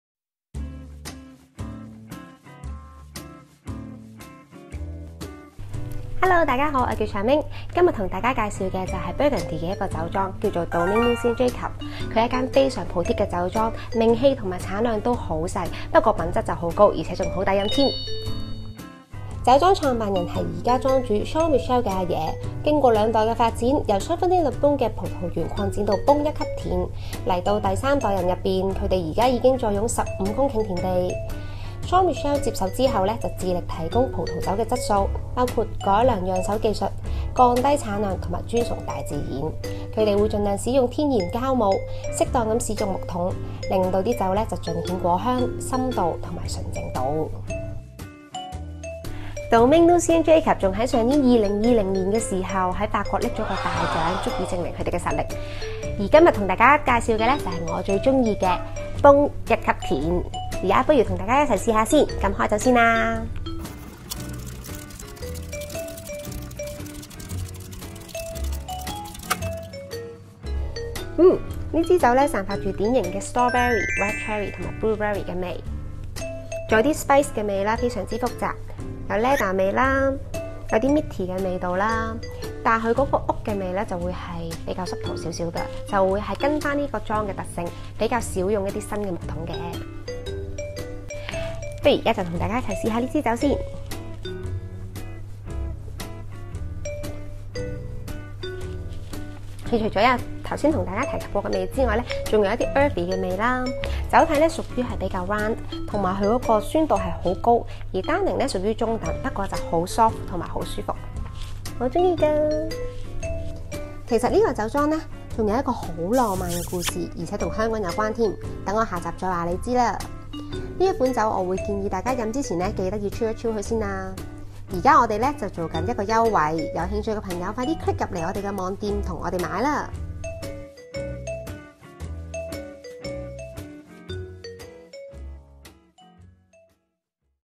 Botshock Tasting Room - Domaine Lucien Jacob, Beaune 1er Cru "Les Cent Vignes" Rouge 2018 | 齊齊品酒
小編Charmaine同你介紹同品嚐Burgundy小眾美酒，而家仲有特價優惠，即時click入嚟睇睇！購買任何產品滿$1,000，仲可即時使用電子coupon，減多$100！(Check Out時輸入PKBALL)

優惠期都3月31曰﹐快啲搶購！